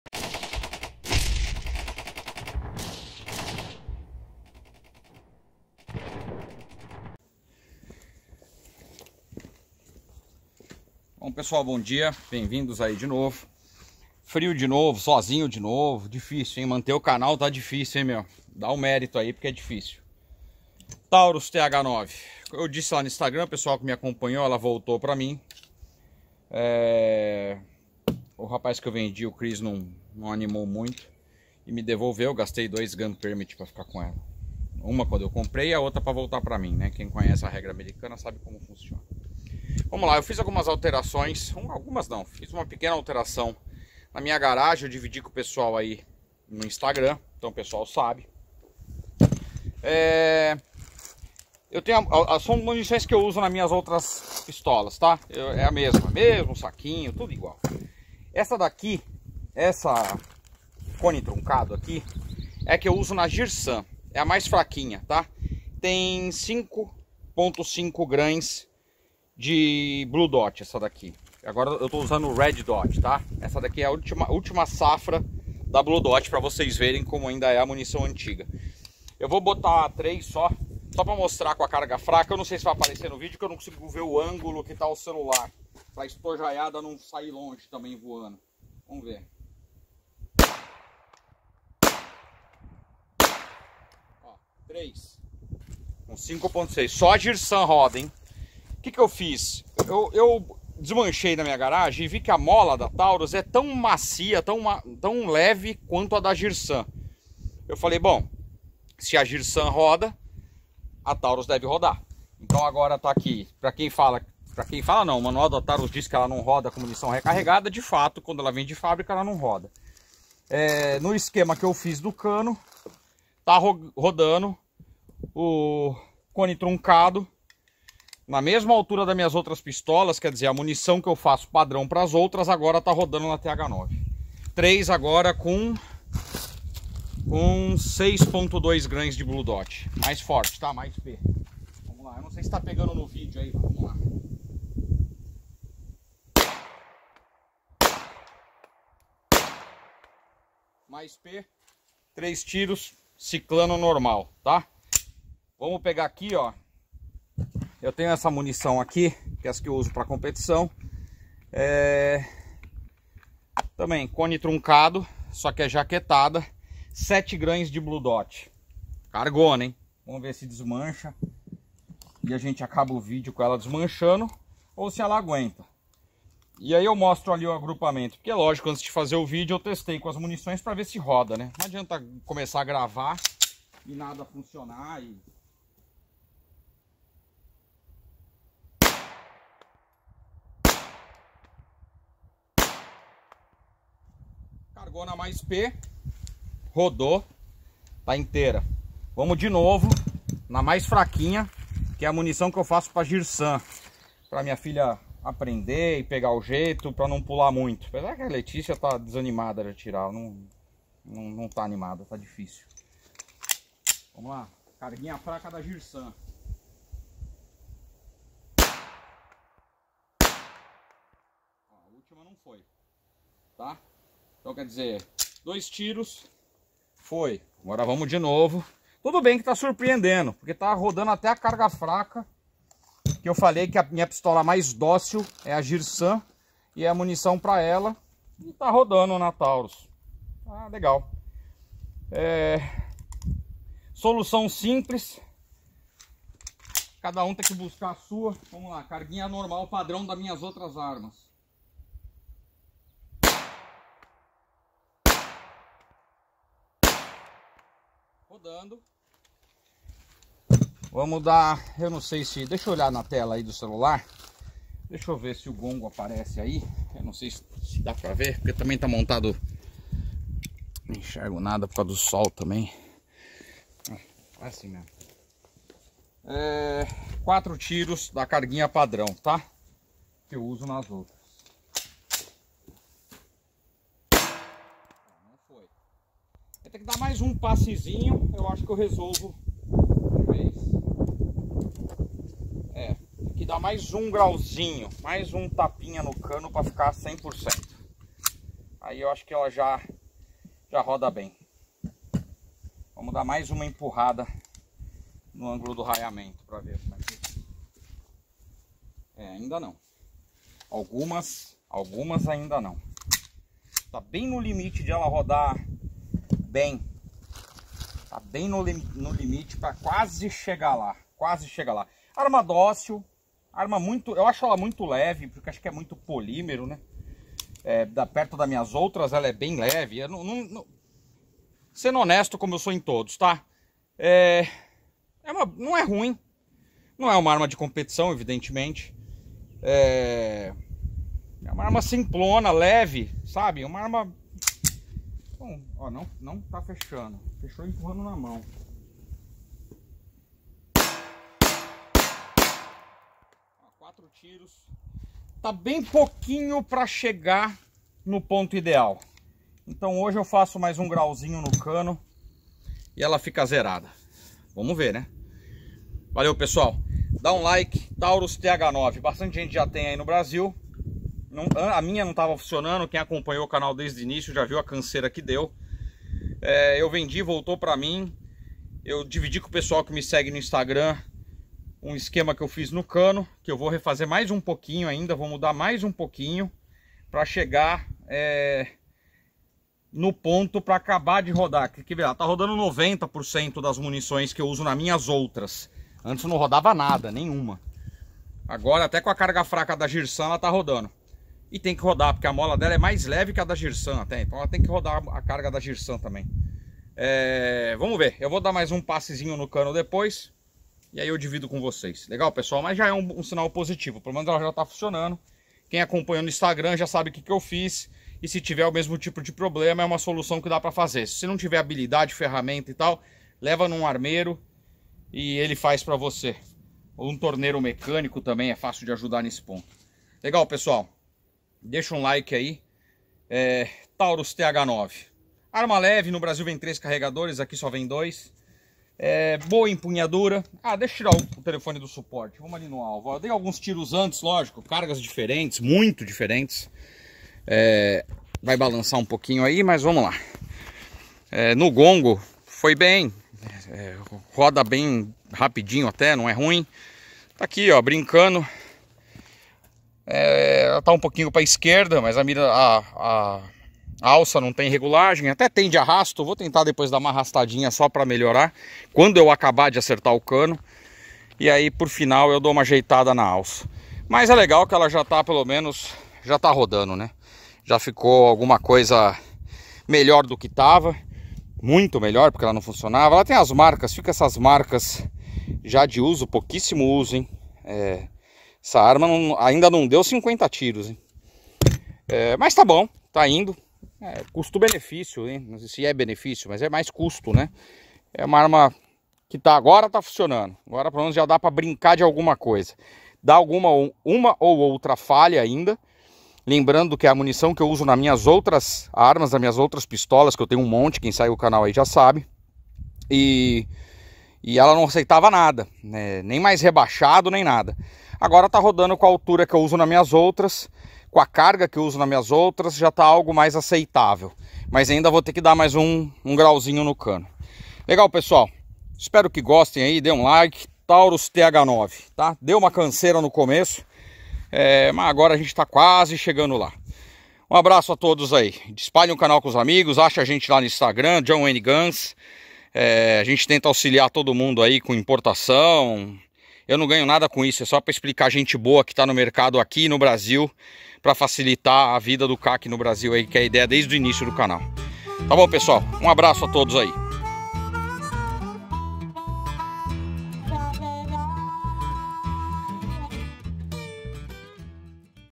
Taurus TH9 9mm
Bom dia pessoal, depois de alguns ajustes no cano ( dividido com o pessoal que nos segue no instagram ) a arma melhorou muito.
Já que antes não rodava nada, e agora esta rodando bem quase todas as munições na altura original ( padrão ).
Espero que gostem e me desculpe as falhas no video, tudo sozinho não é facil.

Abraço a todos, ótimo domingo entre os familiares , boa Páscoa a todos !